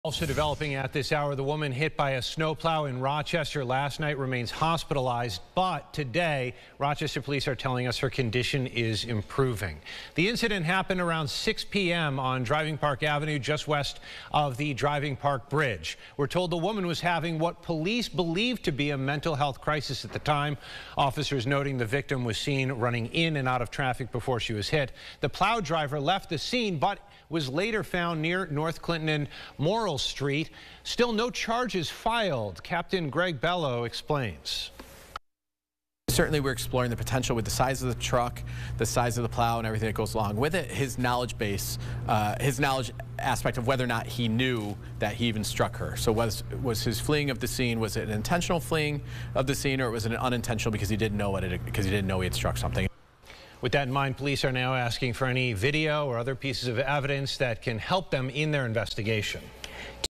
Woman expected to survive after getting hit by snow plow on Driving Park Avenue bridge
A woman is expected to survive after she was hit by a snow plow on the Driving Park bridge Tuesday.